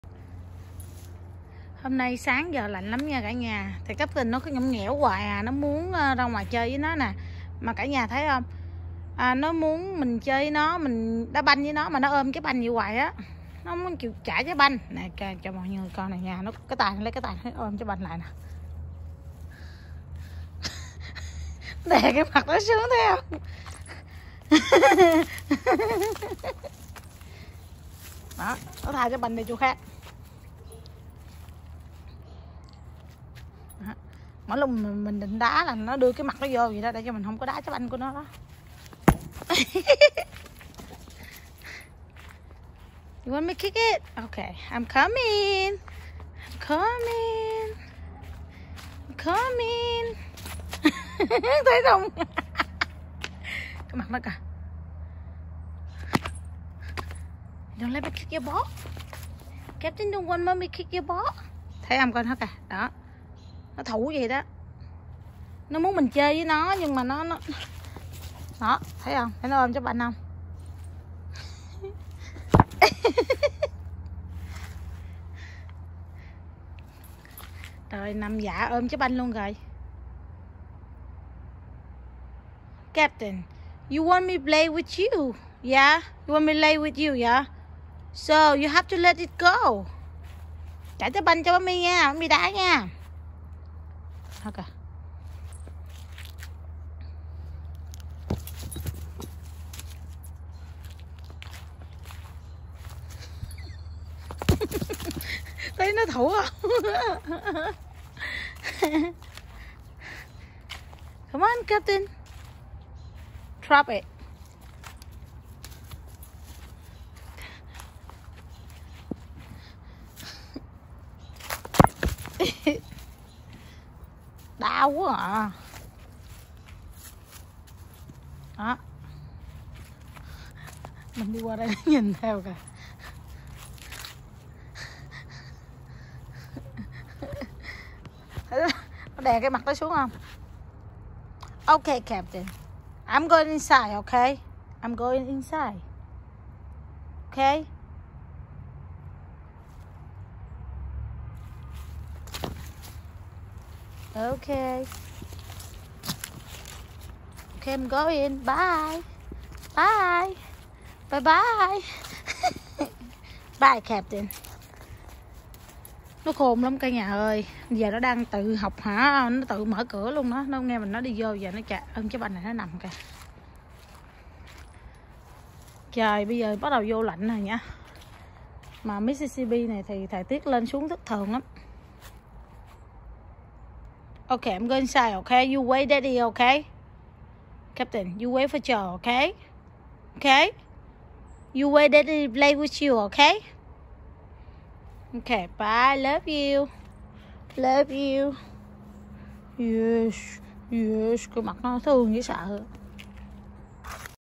Chó con Captain muốn chơi đá banh( cuộc sống ở Mỹ)/ Puppy wants to play football before the dark😂
During the coronavirus quarantine, we adopted a poor little puppy from the shelter, a boxer-mix we named Captain. He is my sweet little baby! He follows us anywhere in the house, even when we shower! He loves to eat everything, including fruits and vegetables and even spicy chili peppers. Every day he grows up and has more interests and hobbies. He talks a lot like a beagle. He chirps like a bird when we don't give him attention. He is so cute, so we made these to keep the memory and share him with you too!

This video shows some of Captain's life.